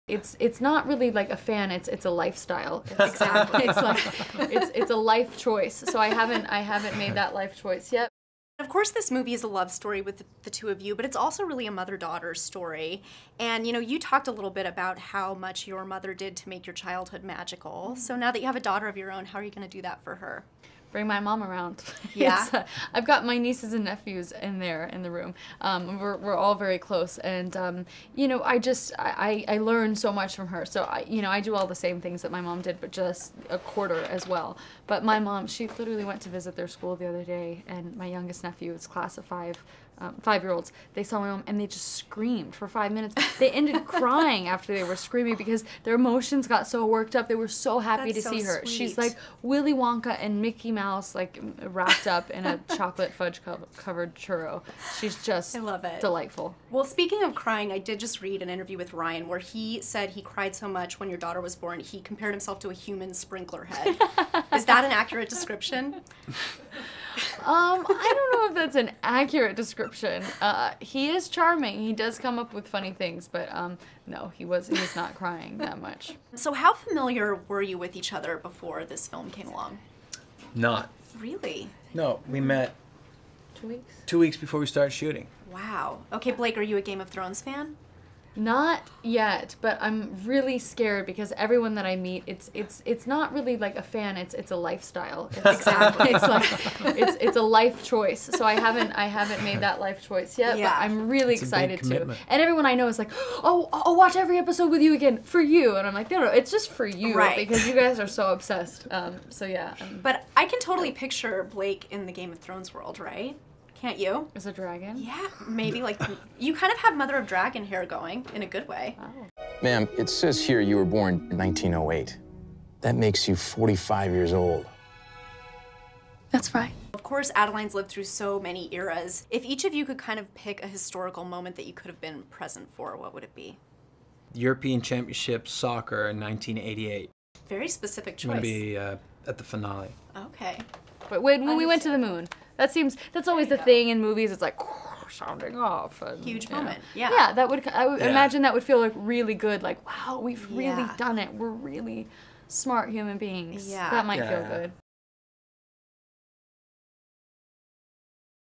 Blake Lively on The Age of Adaline and Ryan Reynolds Crying
Blake Lively stars in the upcoming romantic drama The Age of Adaline as a woman who never grows old, and we chatted with her and her costar, Game of Thrones actor Michiel Huisman, about the movie's magical premise this weekend in LA. Blake also spoke about her newest role as mom to baby James. She said her own mother is her parenting role model and weighed in as to whether or not husband Ryan Reynolds cried quite as much as he said he did when their daughter was born. She also explained us he's "scared" to start watching Michiel's hit HBO show. The Age of Adaline hits theaters April 24.

We just got started on Vine!  Watch us there, too!